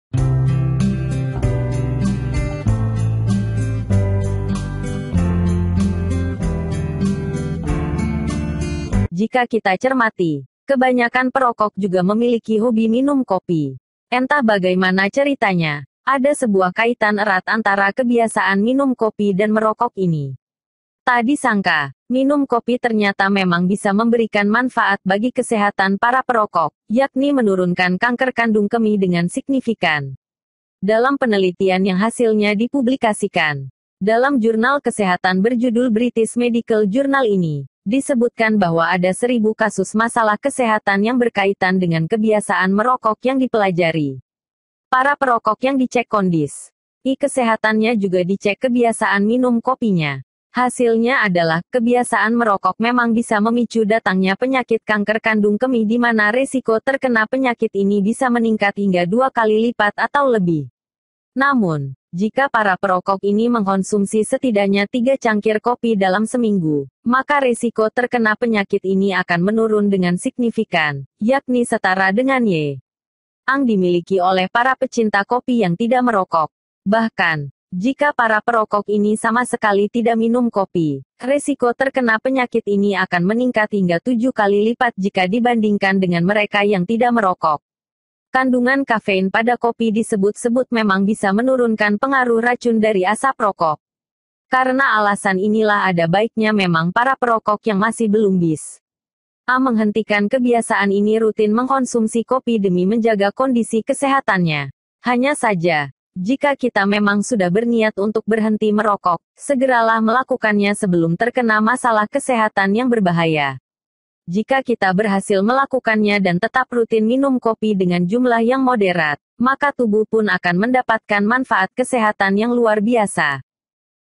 Alasan Mengapa Banyak Perokok Juga Hobi Minum Kopi
kebanyakan perokok juga memiliki hobi minum , kopi. Entah bagaimana ceritanya.